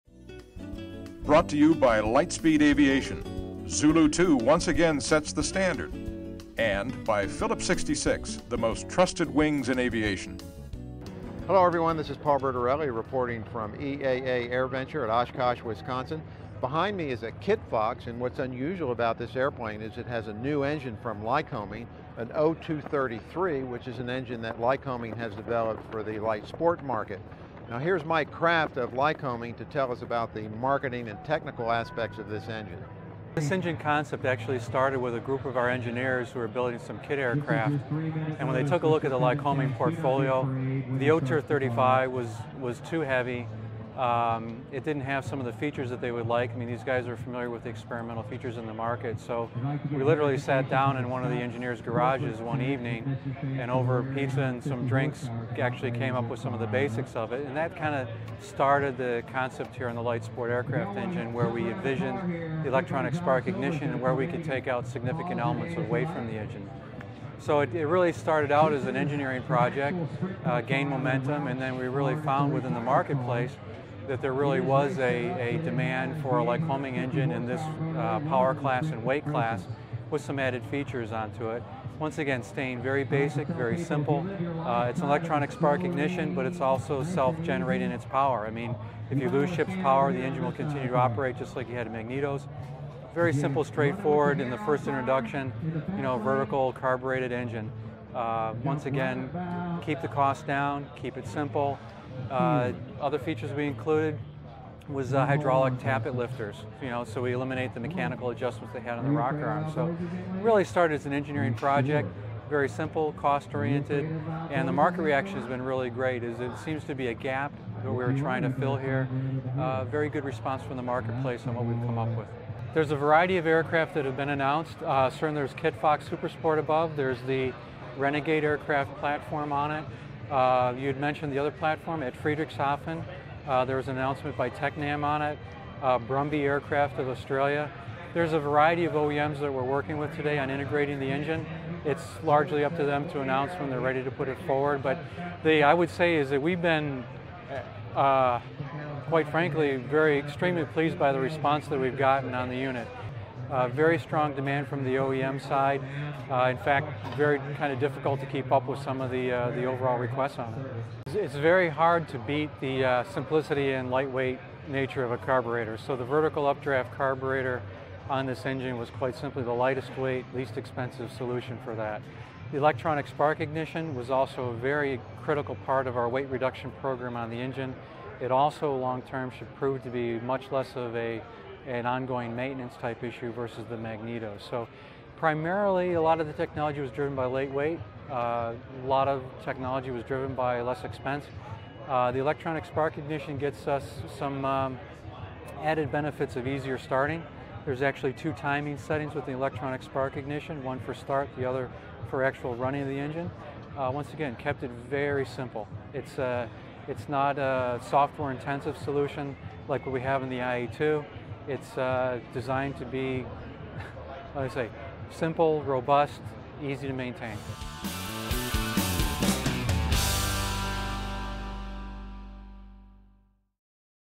Lycoming's New O-233 Takes Flight
At AirVenture 2011, Lycoming staked out its ground in the LSA market by announcing the first installations of the 115-HP O-233 four-cylinder engine. The company's GM Michael Kraft gave AVweb a briefing on the new product.